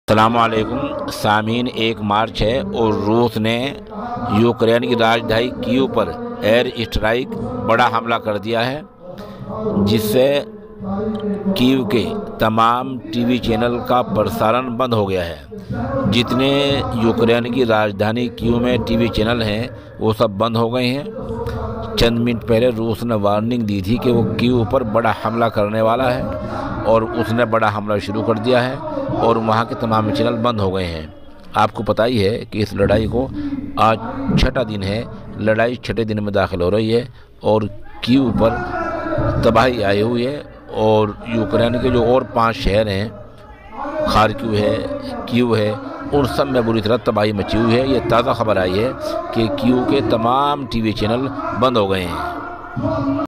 بڑاحملہ۔راج دھانی کے ٹی وی چینل بند Russia Ukraine war
यूक्रेन की राजधानी में बड़ा हमला हो गया है।
वहां के तमाम टीवी चैनल बंद हो गए हैं